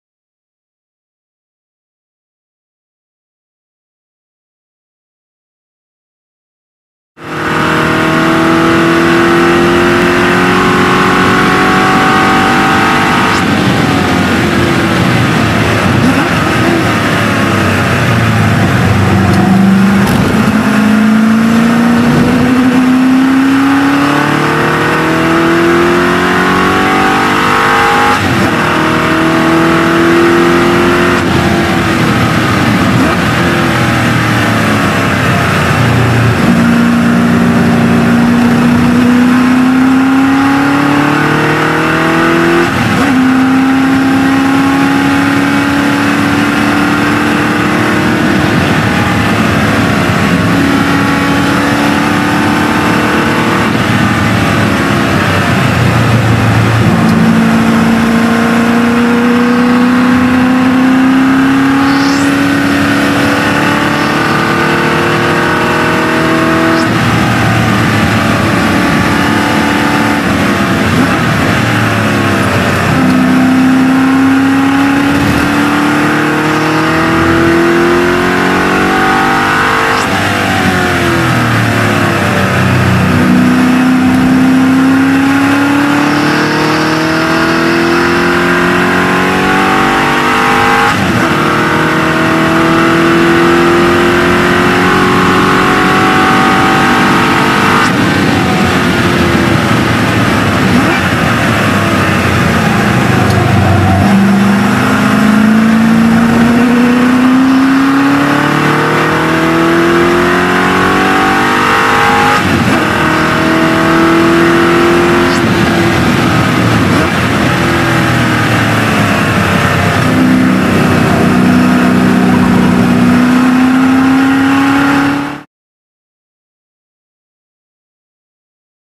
GT40 Donnington 25th November 2008
My GT40 replica at a track day, Donington park.